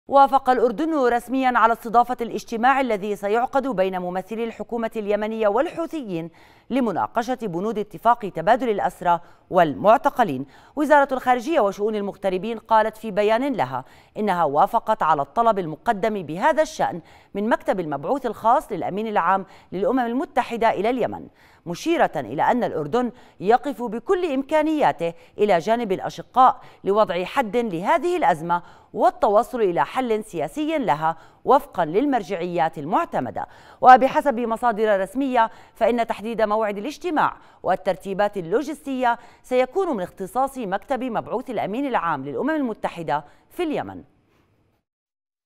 الأردن يستضيف اجتماعا يمنيا - (15-1-2019)
وافق الاردن رسميا على استضافةِ الاجتماع الذي سيعقدُ بين ممثلي الحكومة اليمنية والحوثيين، لمناقشةِ بنودِ اتفاقِ تبادل الأسرى والمعتقلين.